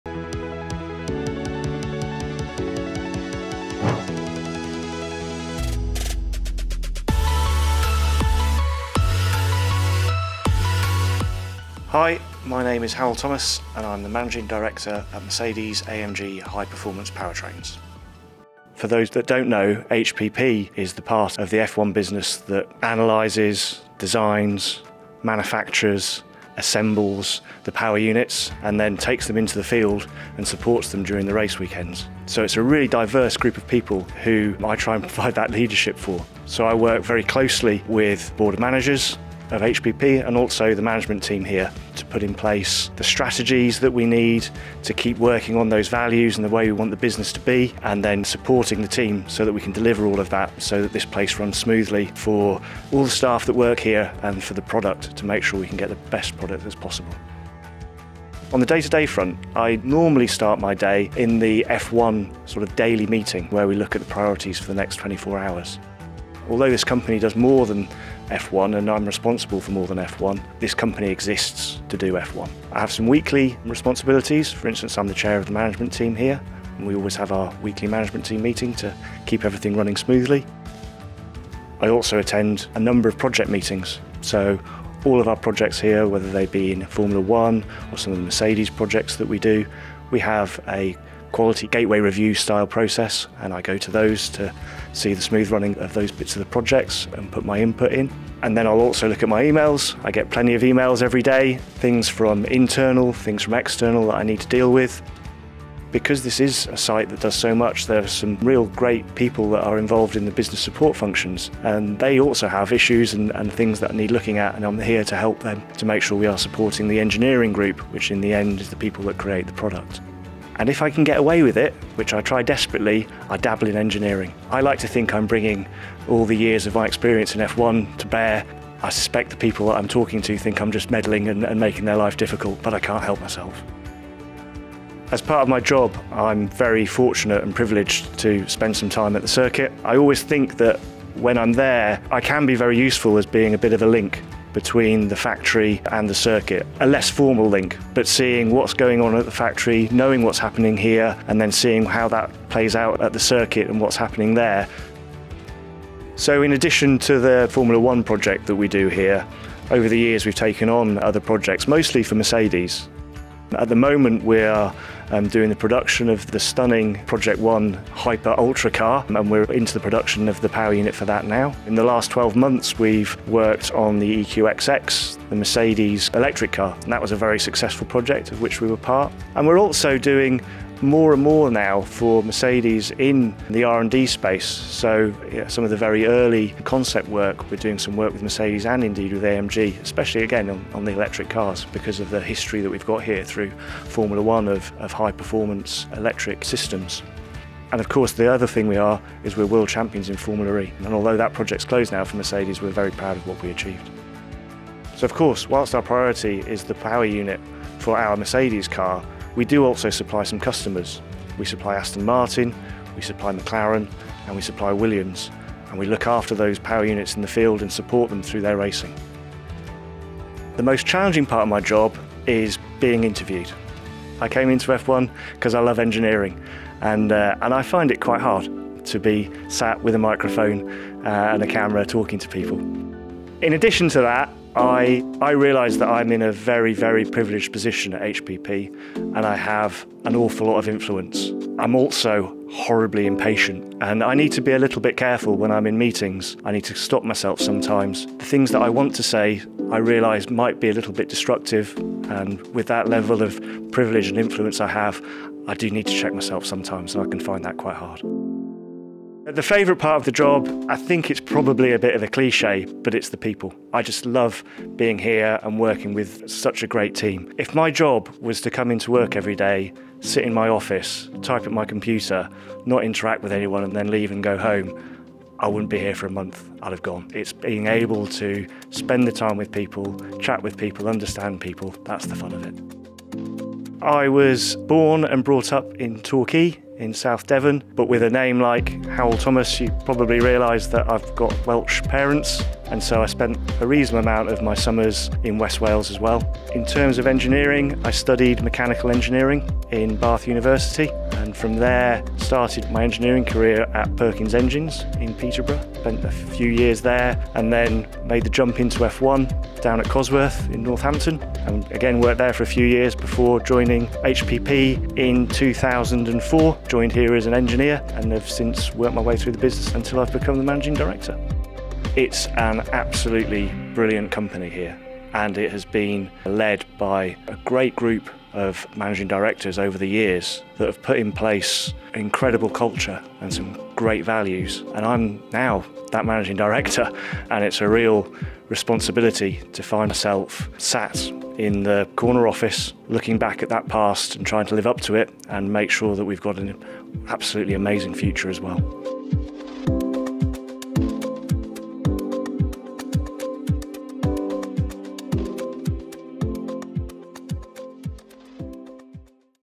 My Job in F1: Hywel | Managing Director, Mercedes AMG HPP
Leading the charge at HPP! 💪 The team in Brixworth design, manufacture, assemble, analyse and run the F1 Power Units.

It’s Hywel’s job, as Managing Director, to support this talented group of people in delivering the best PU possible 👏